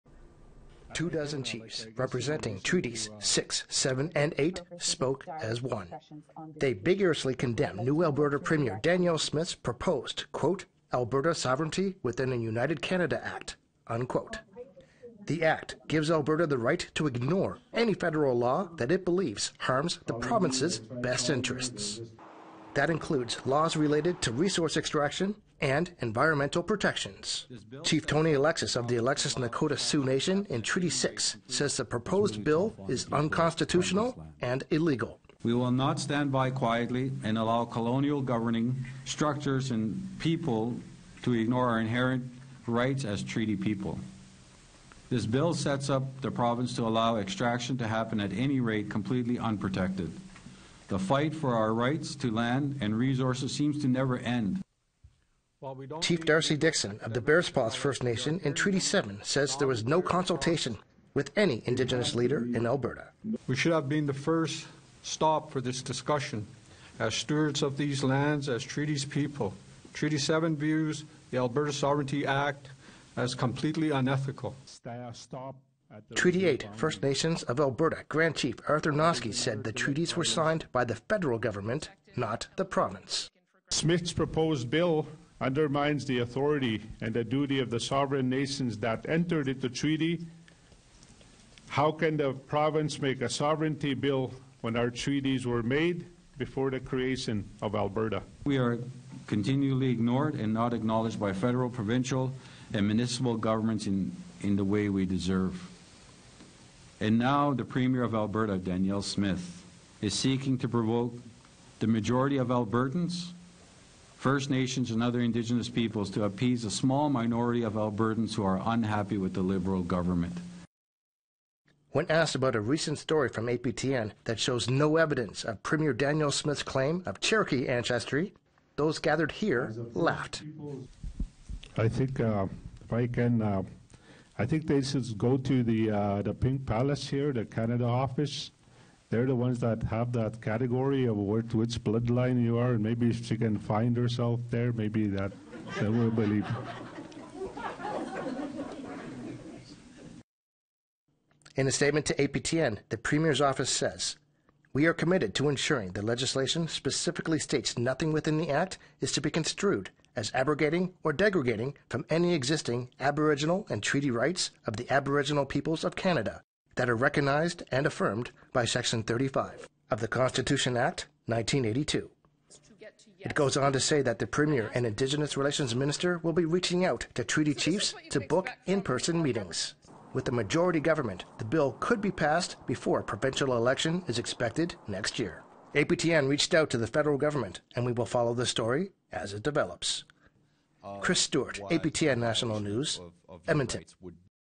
Treaty chiefs in Alberta reject province's proposed sovereignty act | APTN News
All of the treaty chiefs in Alberta are coming together to oppose Premier Danielle Smith’s proposed sovereignty act.

The chiefs held a news conference in Edmonton to challenge the proposed law and declare “it’s offensive and they reject it outright.”